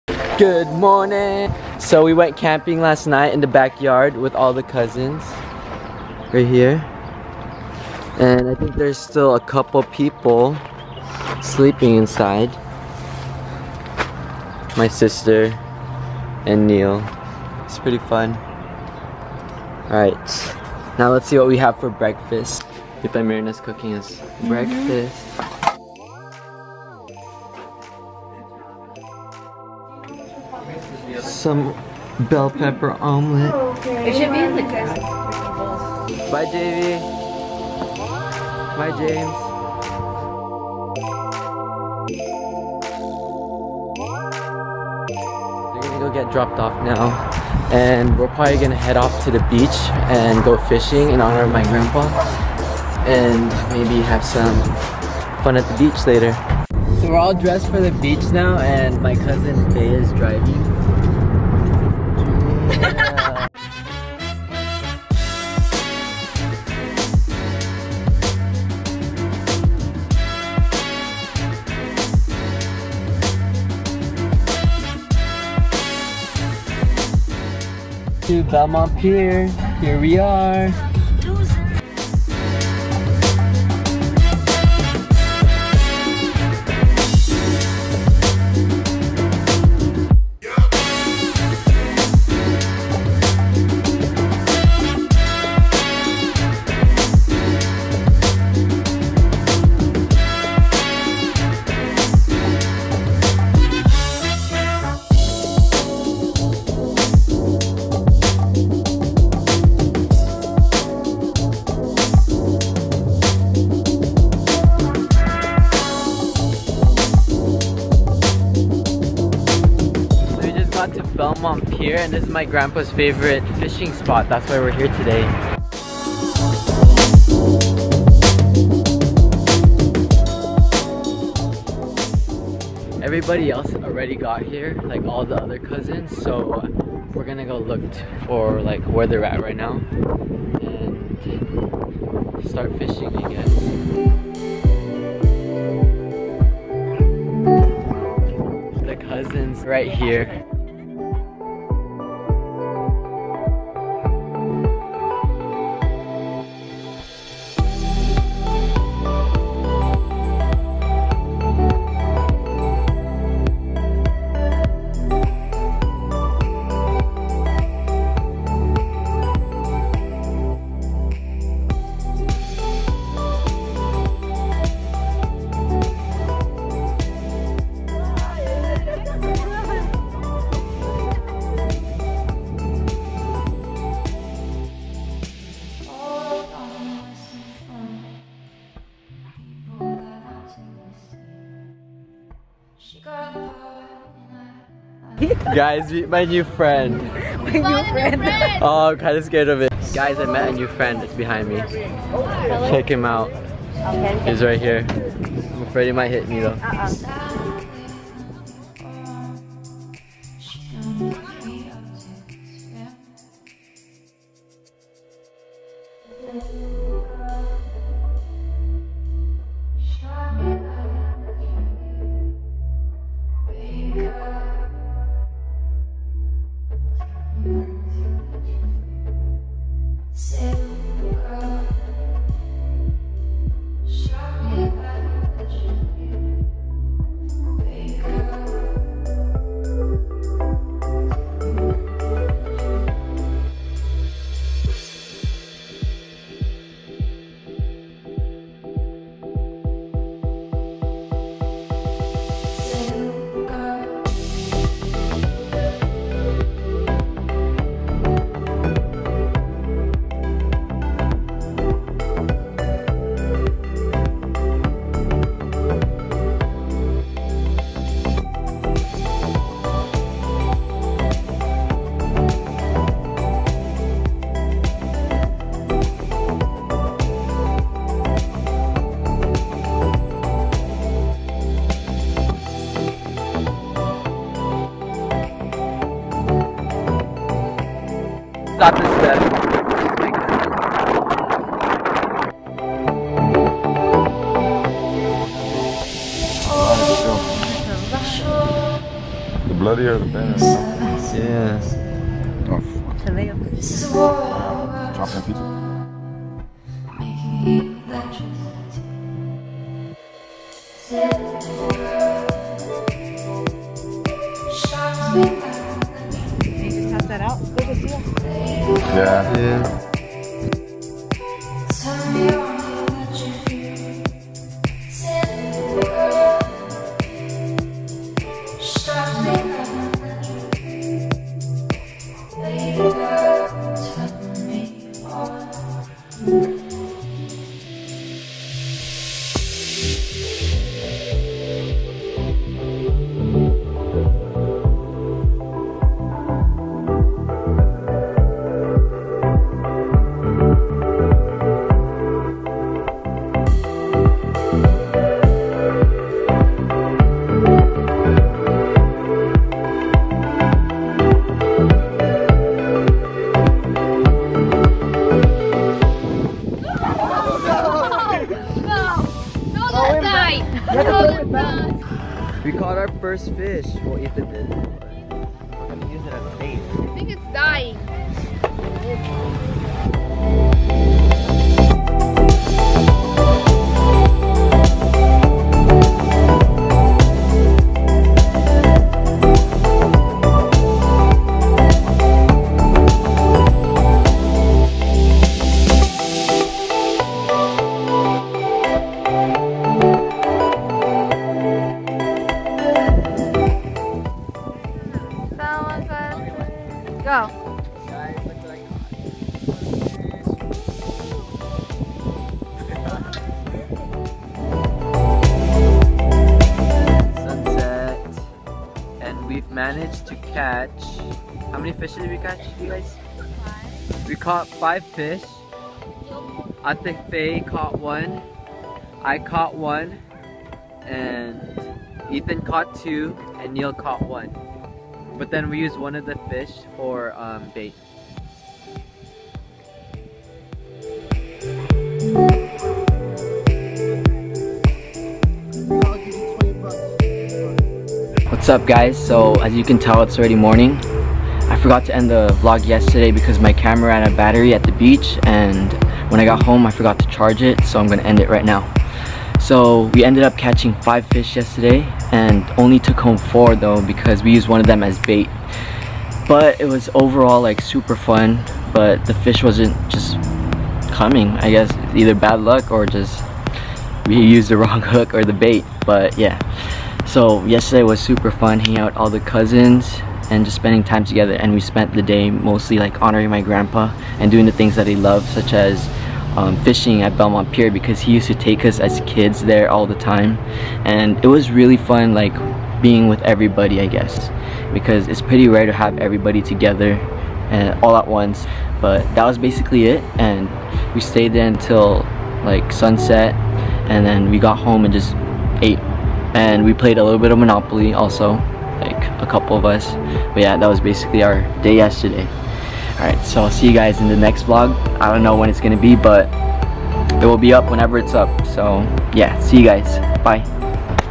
Fishing and Making a New Bird Friend
This is my very first vlog so hopefully I did alright! I'm still learning and trying to get better at talking to the camera so forgive me for not having as much dialogue but i'll try and do a better job next time!

My cousins and I went fishing at Belmont Pier today. We went fishing in honor of my grandpa who passed away recently. Belmont Pier brings back memories of our childhood of when our grandpa used to take us there. Belmont Pier brings back nostalgic memories of when our grandpa first taught us to fish and all the stories that he's told us on that pier. Today was special and it's dedicated to my grandpa. R.I.P. You will never be forgotten! I love you!

Big Thanks To: SYRE, MellowMaswel and Kygo for the music!